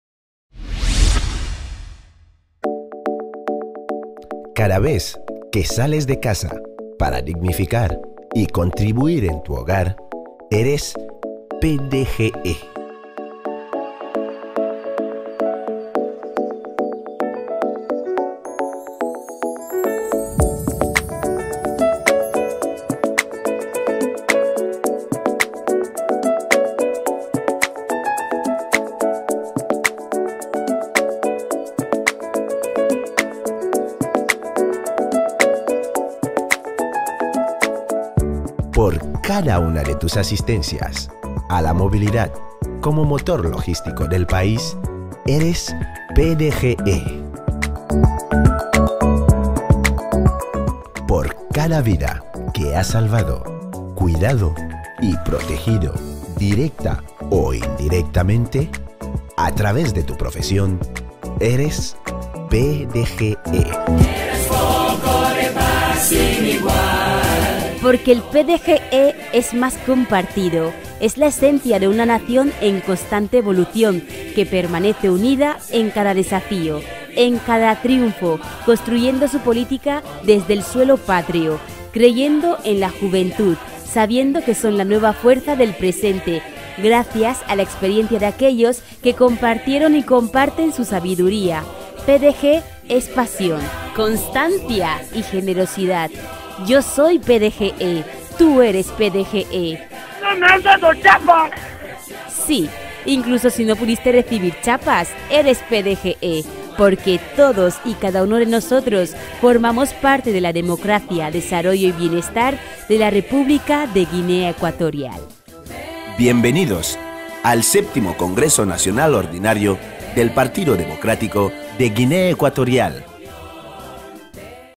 VII CONGRESO NACIONAL ORDINARIO DEL PDGE
Del 22 al 24 de Noviembre, el PDGE celebra su VII Congreso Nacional Ordinario en el Palacio de Conferencias de Ngoló - Bata.

Un evento en el que se analizarán en profundidad cuestiones del ámbito político, económico, y socio-cultural del país.